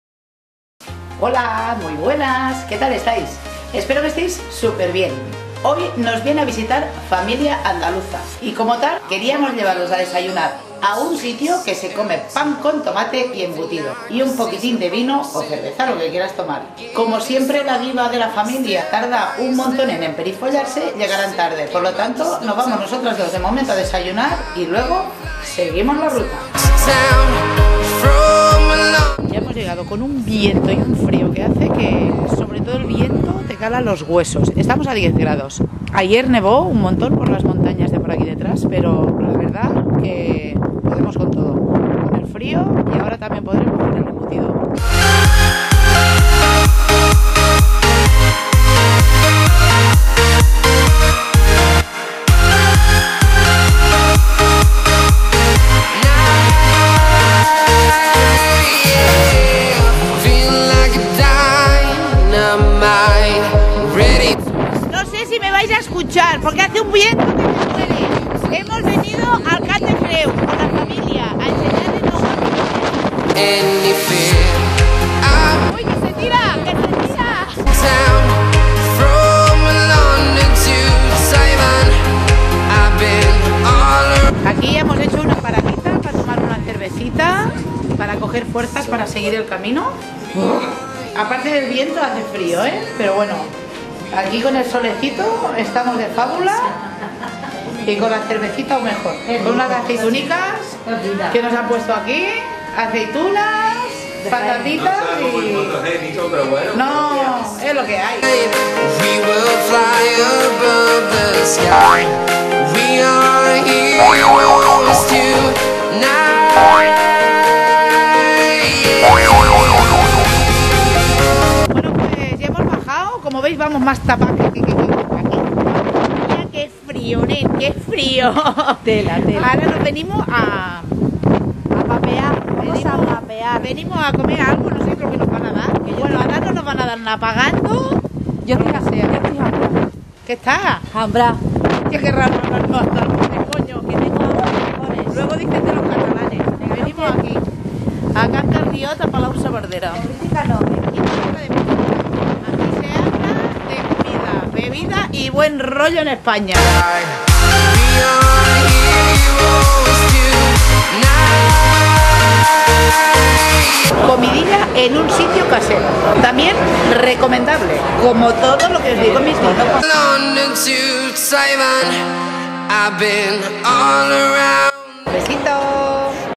DÍA DE VISITA FAMILIAR
Buenas, hoy vamos a pasar un día con visita familiar andaluza. Como siempre, espero que os guste el vídeo. Un besitoooo